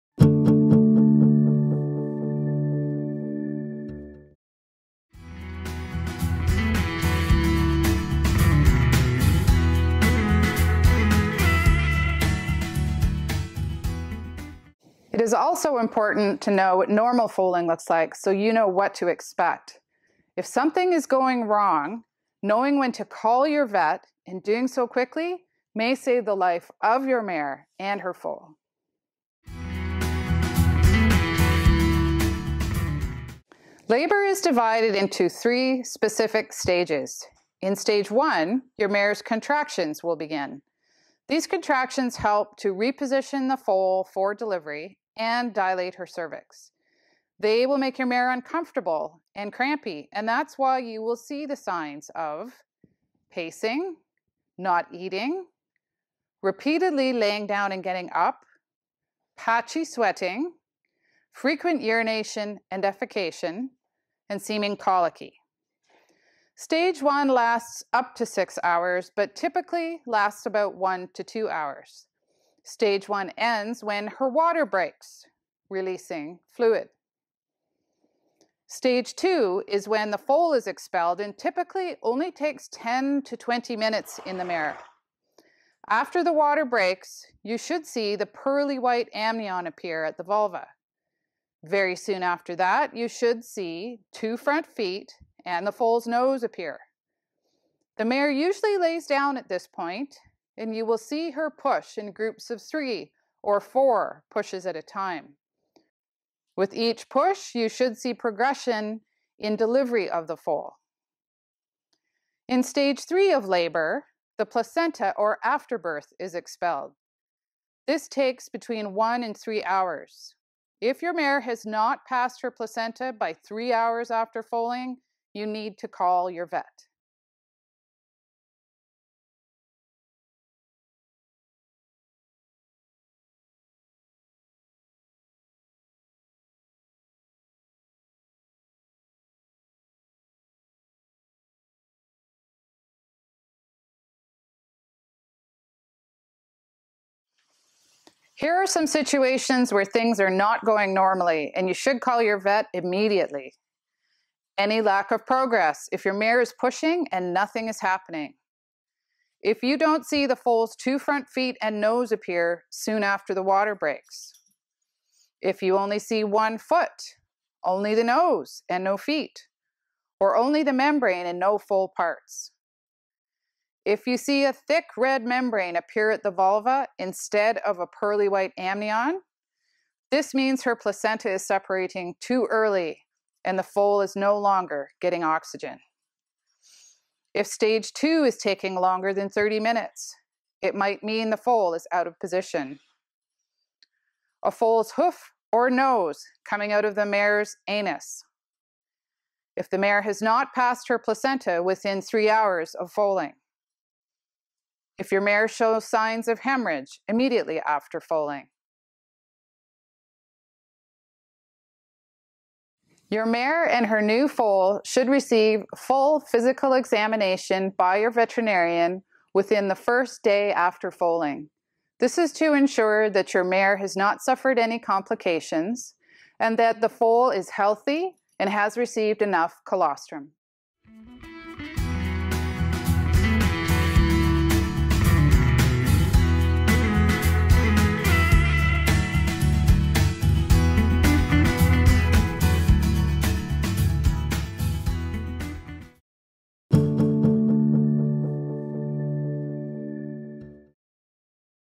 Foaling How To's Monitoring Foaling (2 of 2 "Foaling How-To's" videos)
Dr. Tracey Chenier, an equine reproductive specialist, discusses the stages of labour (foaling) in horses and scenarios where you may need to call your vet immediately.